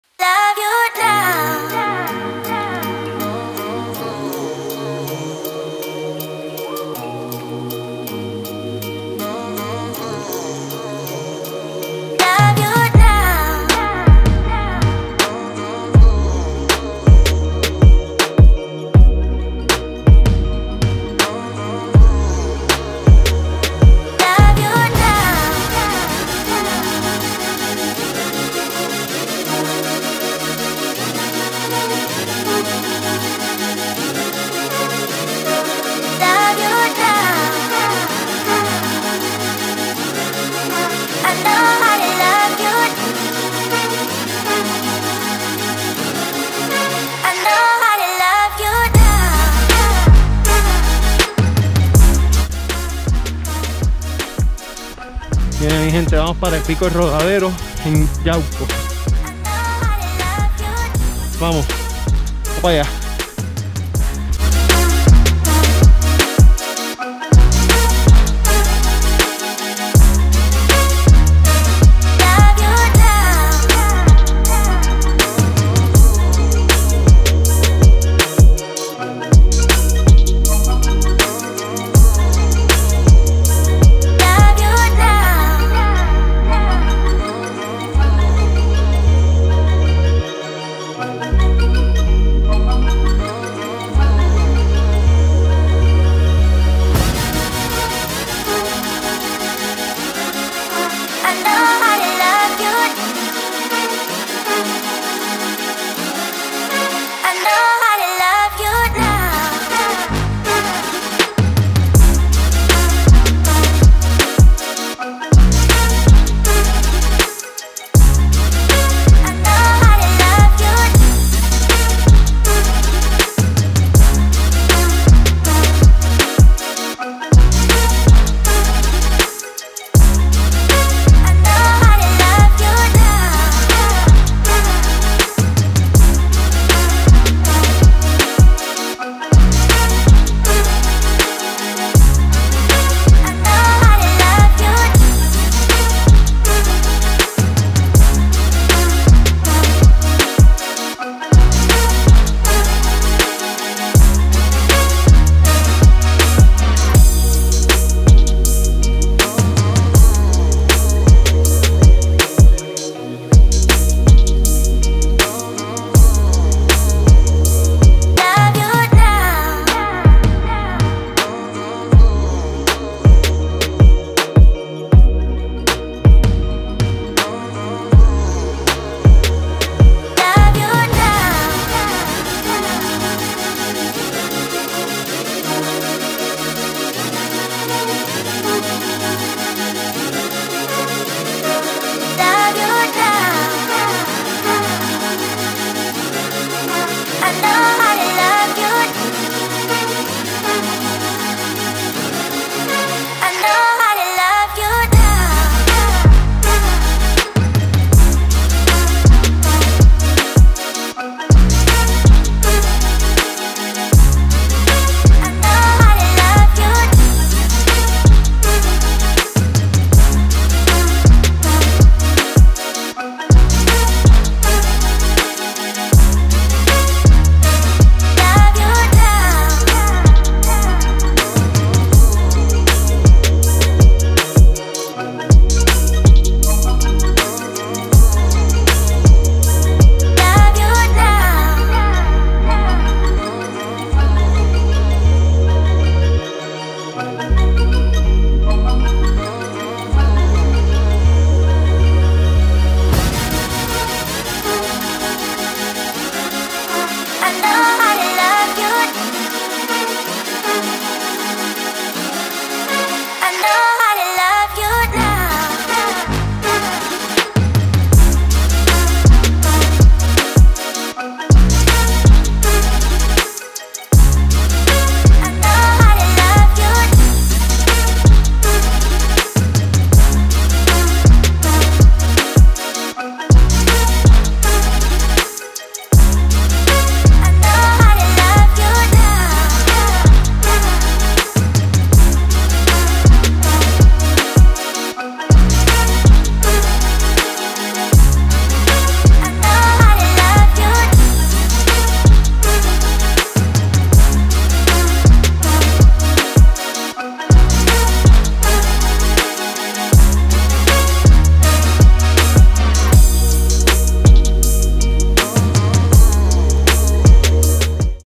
Cerro El Rodadero
Hiking in one of the most beautiful places in PR. It take around 40 minutes to get to the top. The view is amazing and in the rocks there is a small window to the south of the Island.